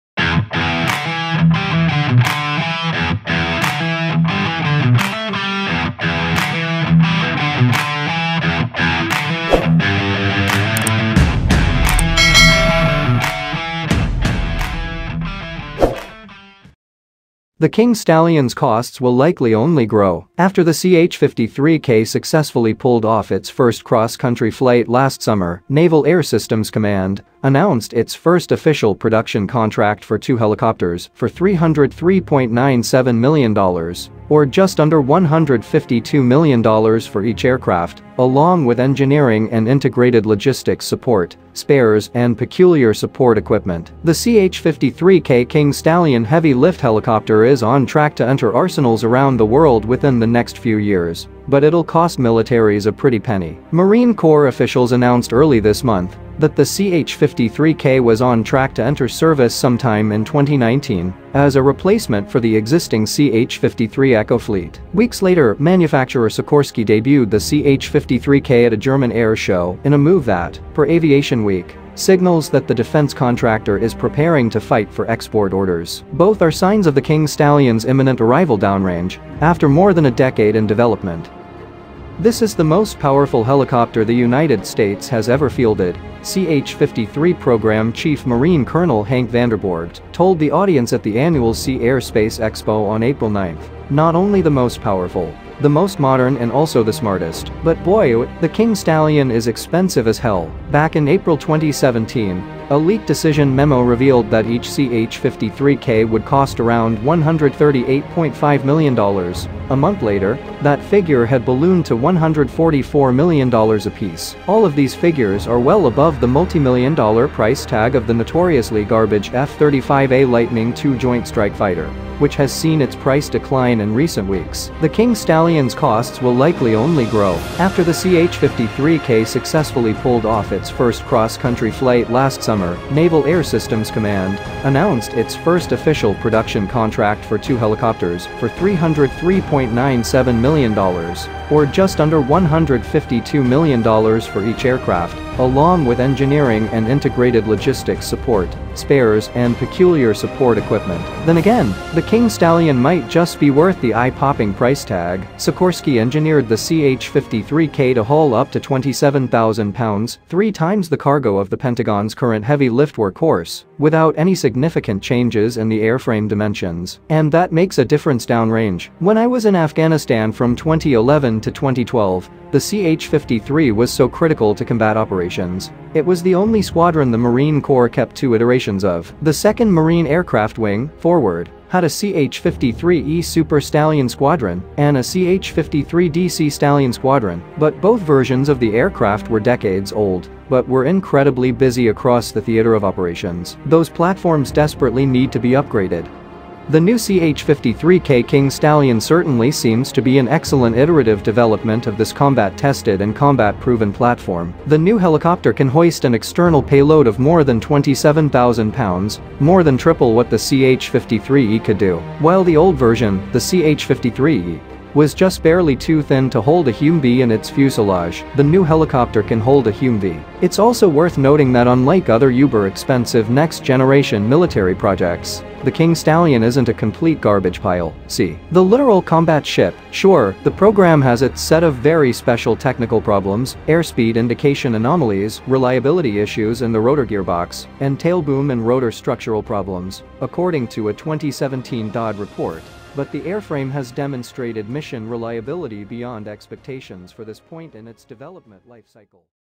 The World's Most Powerful, Most Expensive and Smartest King Stallion (CH-53K) Helicopter
The new CH-53K King Stallion certainly seems to be an excellent iterative development of this combat-tested and combat-proven platform. The new helicopter can hoist an external payload of more than 27,000 pounds, more than triple what the CH-53E could do. While the old version, the CH-53E, was just barely too thin to hold a Humvee in its fuselage, the new helicopter can hold a Humvee.

It’s also worth noting that unlike other uber-expensive next-generation military projects, the King Stallion isn’t a complete garbage pile (see: the Littoral Combat Ship. Sure, the program has its set of very special technical problems  airspeed indication anomalies, reliability issues in the rotor gearbox, and tail boom and rotor structural problems, according to a 2017 DoD report — but the airframe has demonstrated mission reliability beyond expectations for this point in its development lifecycle.